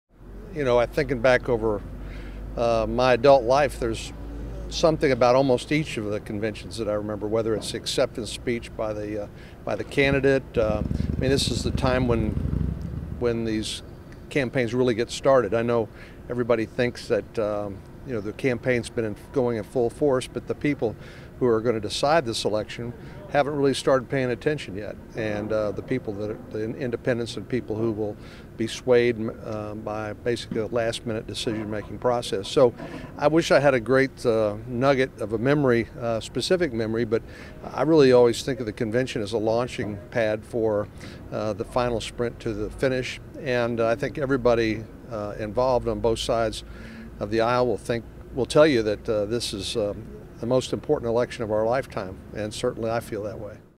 U.S. Sen. John Cornyn: 'This is the most important election of our lifetime.'
U.S. Sen. John Cornyn (R-Texas) "campaigns really get started" after the political nominating conventions.